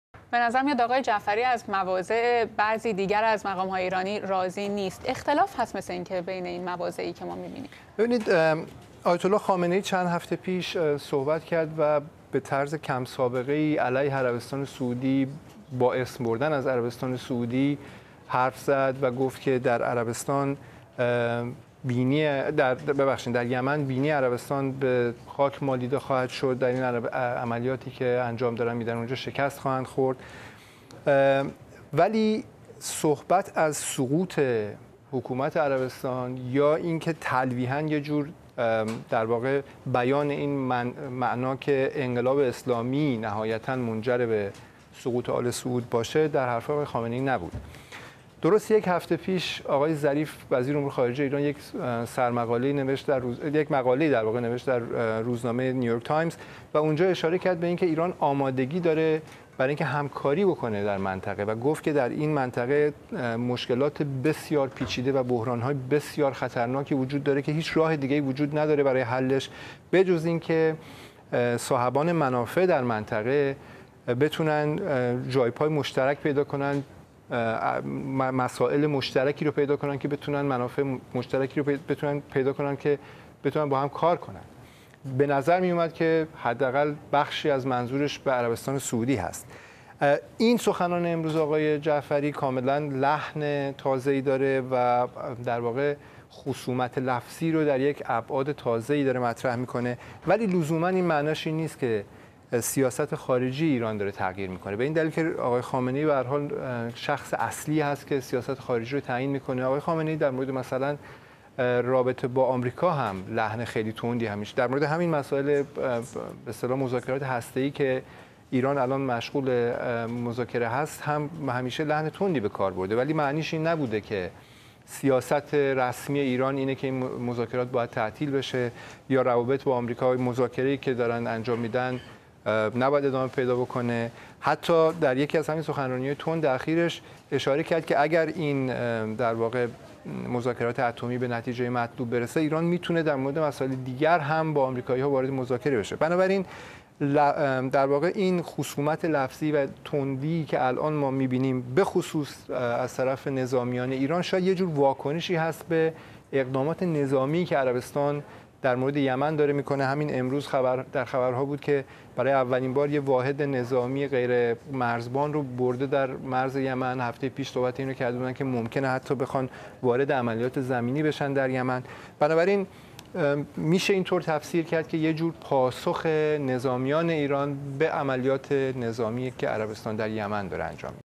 'سپاه از اقدامات نظامی عربستان به خشم آمده است'
فرمانده سپاه پاسداران انقلاب اسلامی در ایران گفته است که "موج بعدی تحولات منطقه منجر به سقوط آل‌سعود خواهد شد." امید پارسانژاد از سایت بی‌بی‌سی فارسی می‌گوید که چنین اظهاراتی از طرف نظامیان، لزوما به معنای این نیست که سیاست خارجی ایران تغییر خواهد کرد.